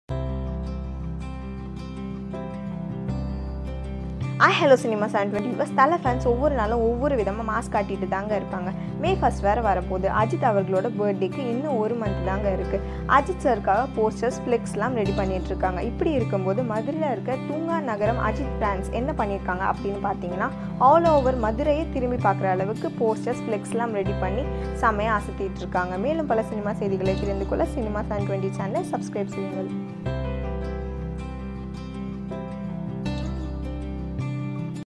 MADURAI AJITH FANS START THEIR CELEBRATION
மதுரை தூங்காநகரம் அஜித் ரசிகர்கள் மதுரையையே திரும்பி பார்க்க வைத்த போஸ்ட்டர்